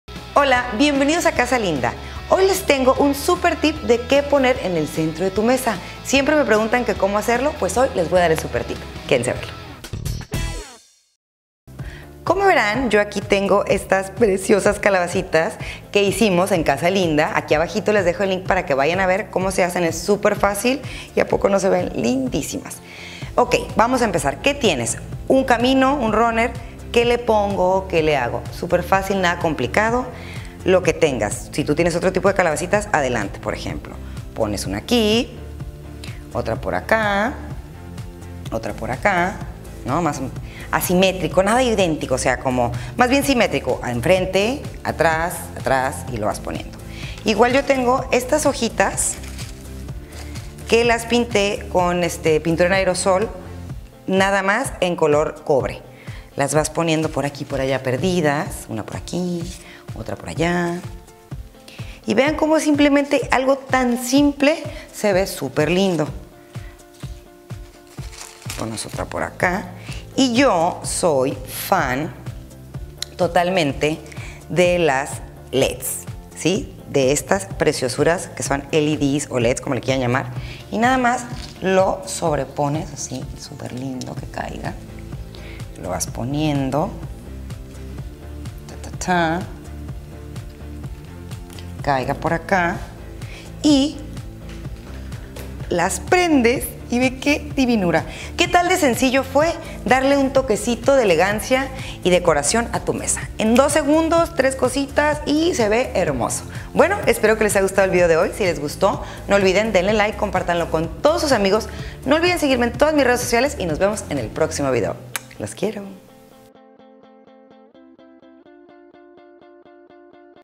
CENTRO DE MESA OTOÑAL/FALL CENTER PIECE
Te muestro lo fácil y rápido que es poner linda tu mesa!.